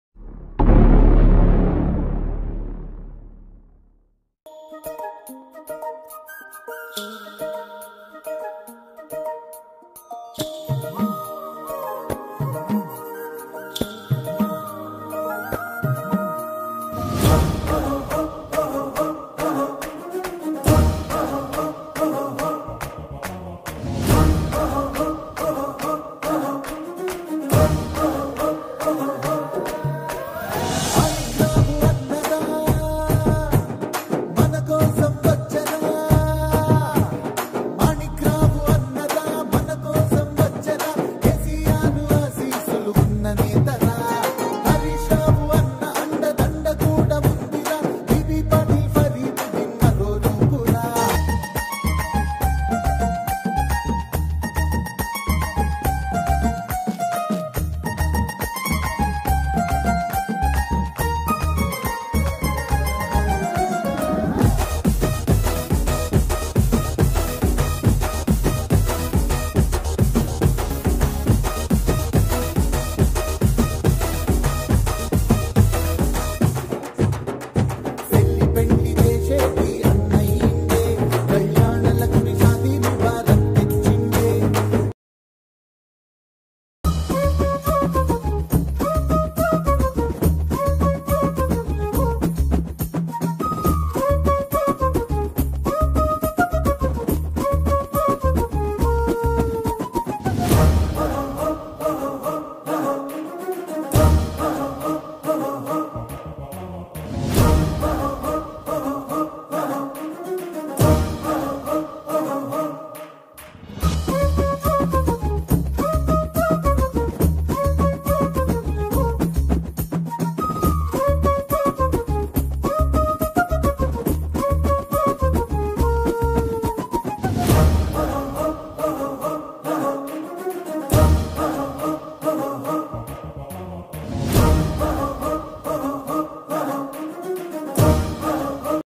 మ్యాతరి వారి వివాహ కార్యక్రమానికి హాజరైన జహీరాబాద్ ఎమ్మెల్యే కె.మాణిక్ రావు గారు | Mukkani
మ్యాతరి వారి వివాహ కార్యక్రమానికి హాజరైన జహీరాబాద్ ఎమ్మెల్యే కె.మాణిక్ రావు గారు | కొహీర్ | Mukkani Brothers

Hi Friends, Welcome to Mukkani Brothers, Please Support us and do the following.